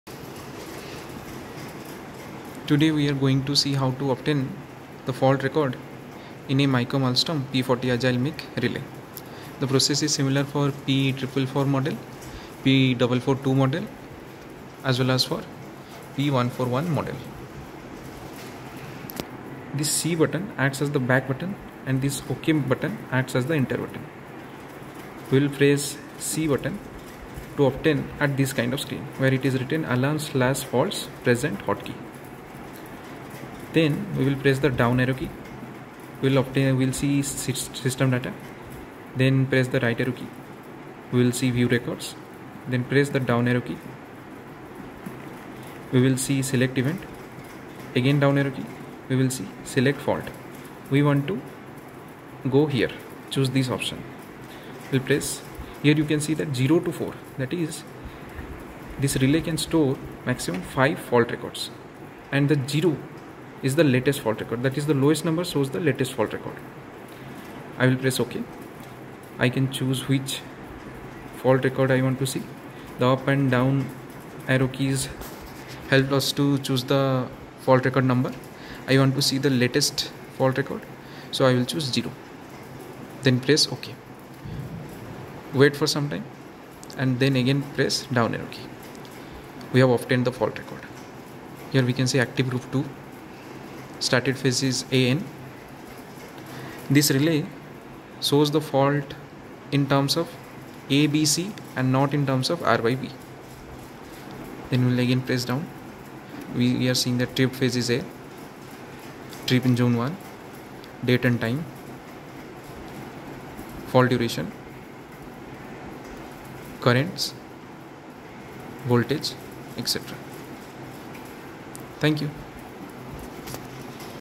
Micom ALSTOM DP Relay | P40 Agile | Fault Record viewing process.
The process is similar for P444, P442, P141 and similar Micom ALSTOM Relays.